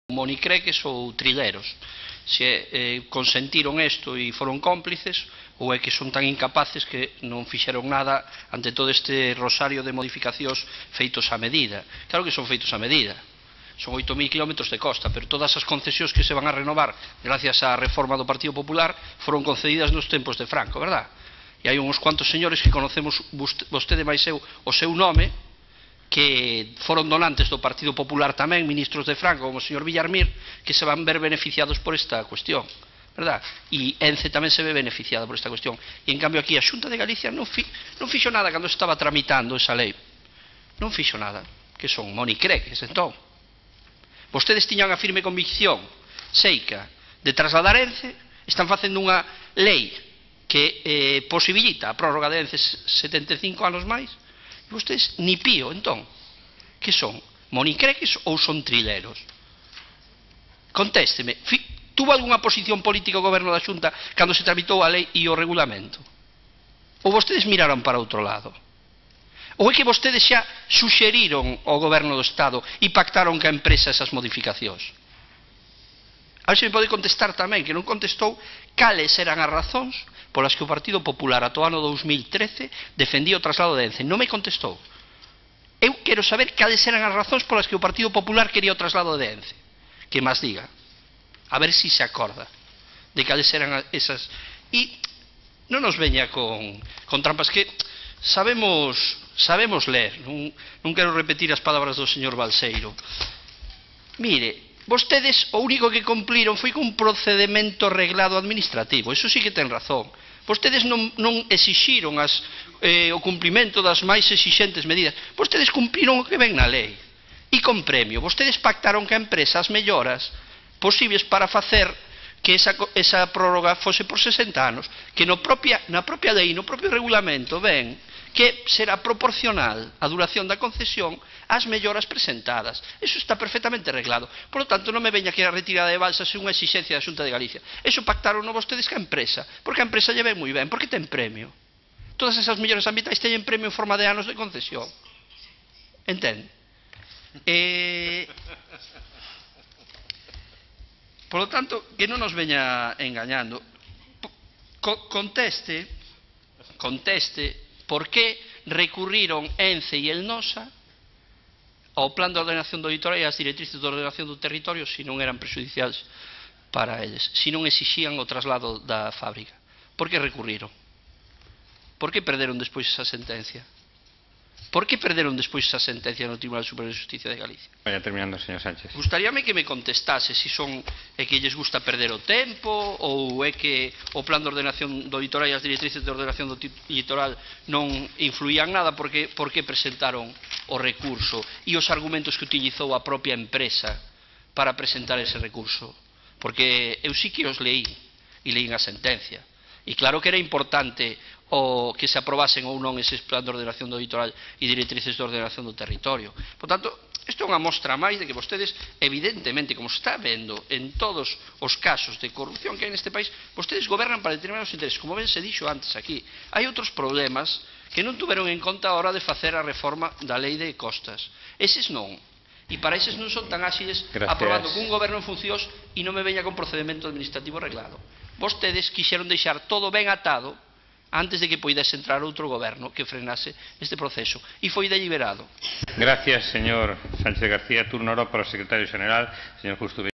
"Quixeron todo ben atado con ENCE antes de que entrase outro goberno e frenase o proceso"!
O viceportavoz de AGE, Antón Sánchez, acusou ao PP de actuar de xeito premeditado para deixar todo ben atado respecto da permanencia de ENCE na ría de Pontevedra durante 60 anos máis. Sánchez esixí ao goberno galego que explique cales eran as razón do Partido Popular para apoiar ata o ano 2013 o traslado de ENCE e que ten cambiado para que agora actúen de cómplices da empresa facilitando a súa permanencia "esixindo únicamente o que pon a lei e ainda por riba con premio".